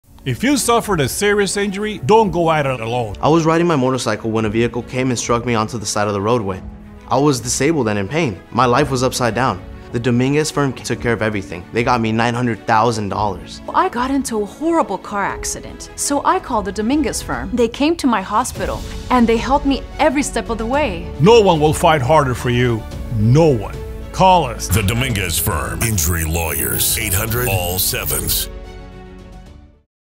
If You Suffered a Serious Injury, You Can’t Afford to Go It Alone
If you were seriously injured in an accident, don’t take on the insurance companies by yourself. The Dominguez Firm has almost 35 years of experience successfully handling all kinds of serious accident cases. These include auto, pedestrian, bus and motorcycle accidents. Let us put our resources and knowledge to work for you.

--Attorney J.J. Dominguez